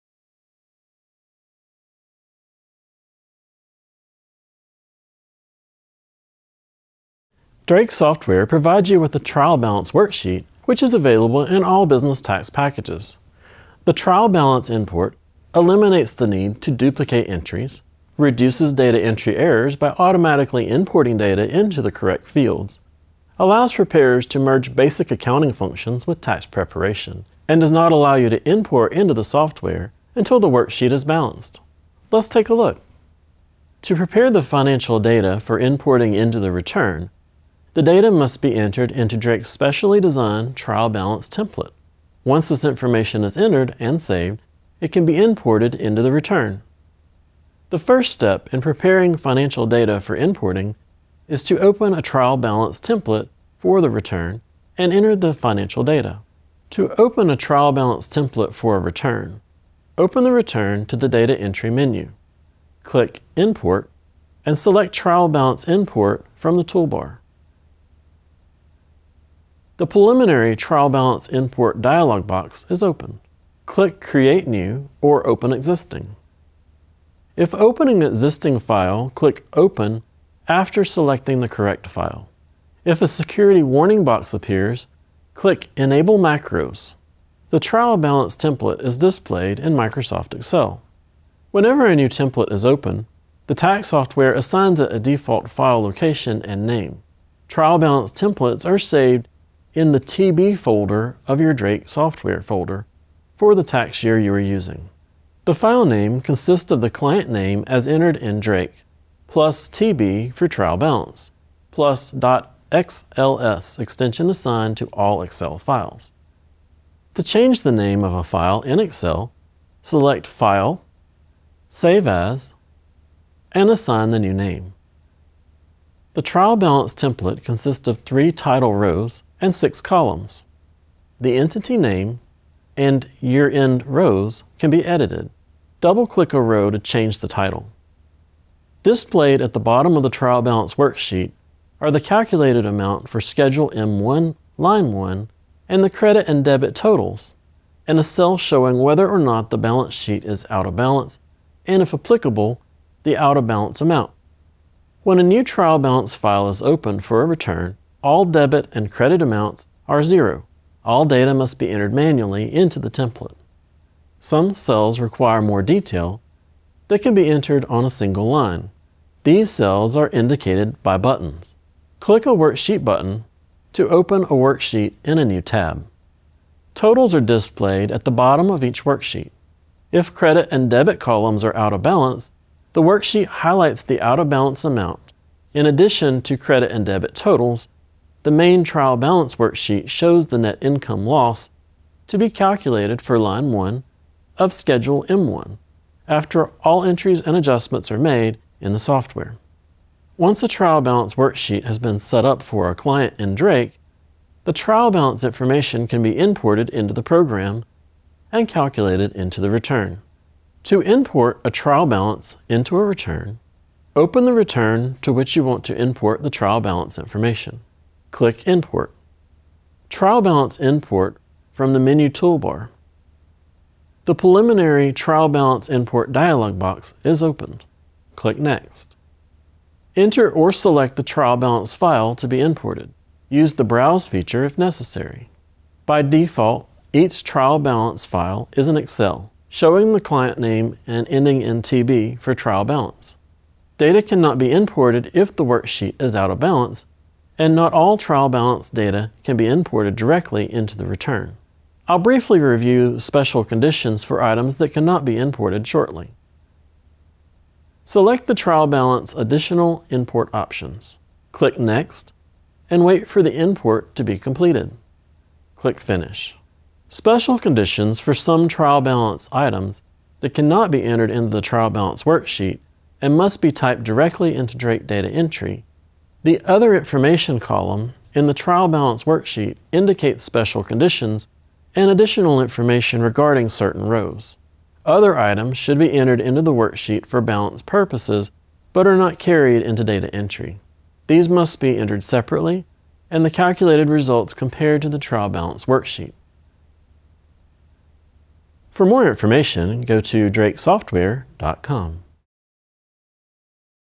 Trial Balance Import
How to use the Trial Balance worksheet that is included in all Drake Tax´s tax packages.